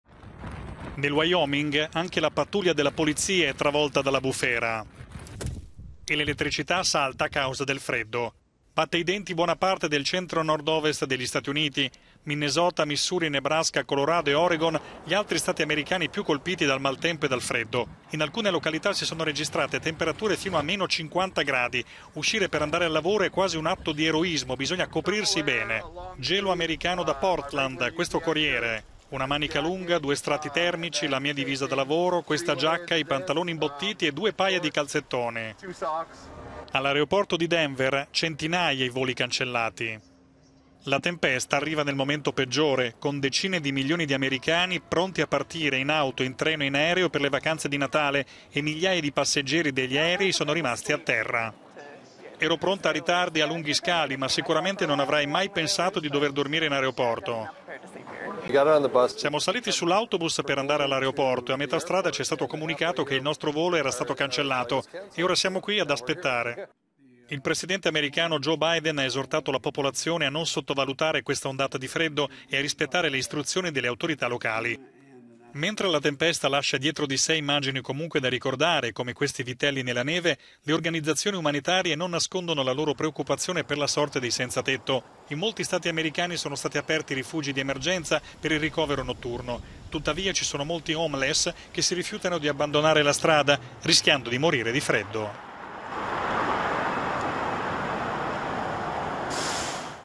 Gelo americano: gli Stati Uniti battono i denti per il freddo
Ondata di freddo glaciale nel centro-nord-ovest degli Usa: anche 50 gradi sotto zero, in alcune località! Preoccupazione delle associazioni umanitarie per i senzatetto: in molte città, sono stati istituiti rifugi di emergenza per il ricovero notturno degli homeless